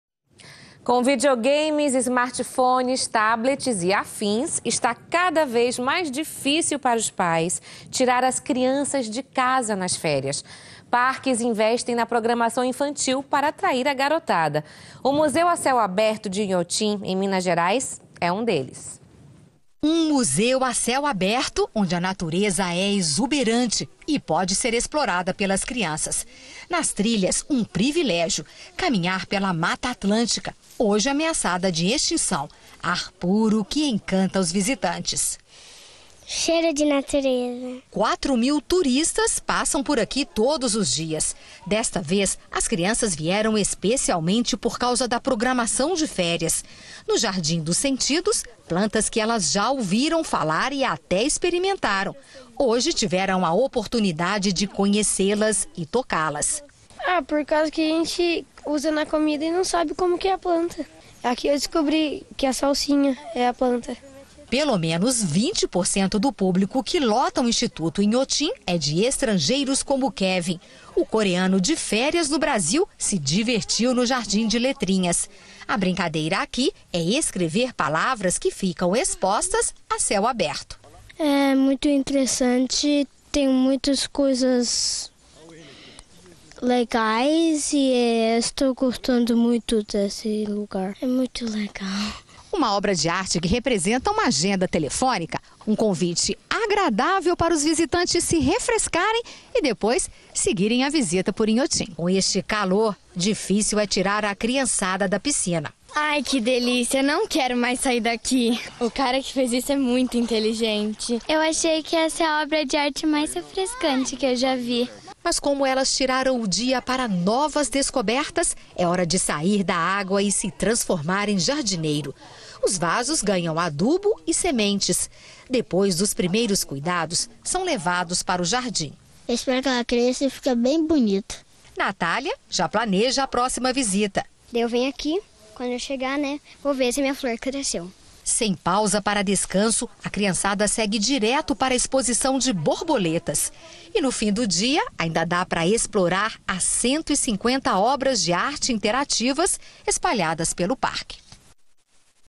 REPÓRTER VANDA SAMPAIO - FÉRIAS INHOTIM
A REPORTAGEM MOSTRA A PROGRAMAÇÃO DE FÉRIAS ESPECIAL PARA CRIANÇAS EM INHOTIM. O MOVIMENTO CHEGA NO LOCAL  A CINCO MIL PESSOAS POR DIA NO LOCAL.